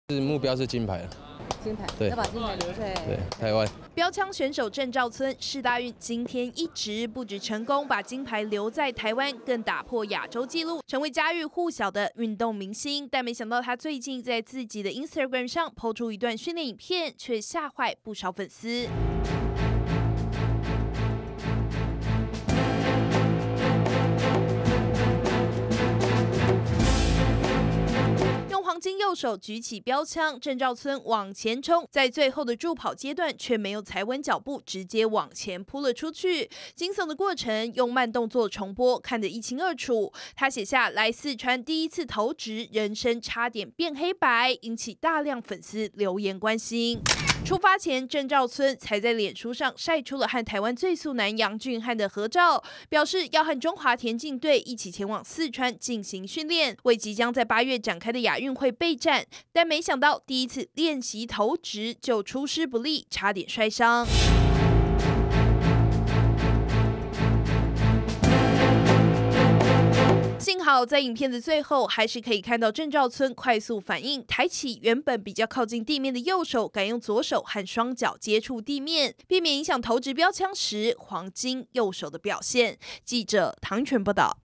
20180325中天新聞 「黃金右手」險摔傷 鄭兆村：人生差點變黑白
世大運標槍金牌選手鄭兆村，一舉一動都備受外界矚目，他最近到大陸四川，進行一個月的移地訓練，沒想到竟然差點發生意外，他在自己的Instagram上，PO出事發當時的影片，感嘆寫下「人生差點變黑白」，引起不少粉絲關心。